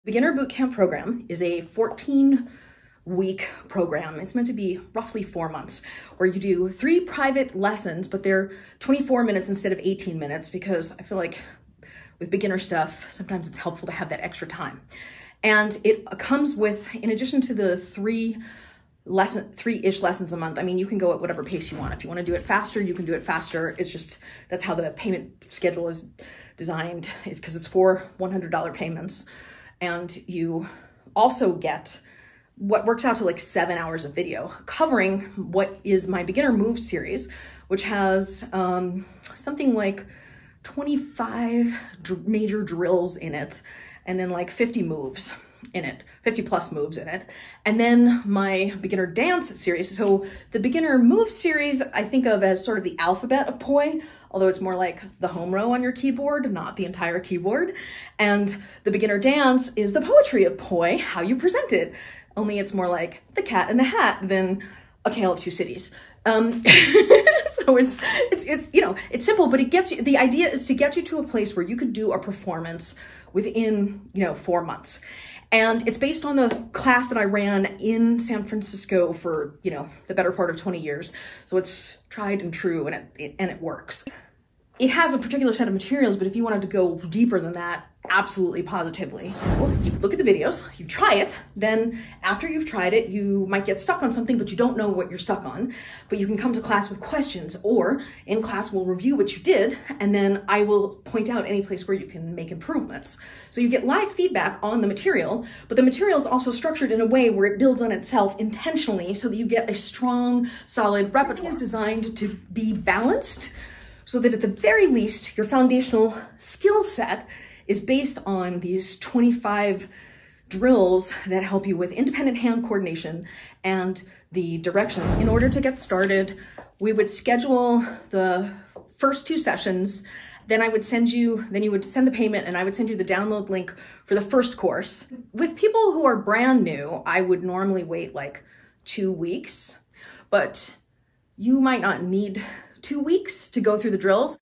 Beginner Poi Performance Boot Camp over Zoom
This video discusses the structure of the Temple of Poi Beginner Poi Performance Boot Camp, a program designed to help new poi artists go from no or low skill to their first performance in ~4 months the written details of which can be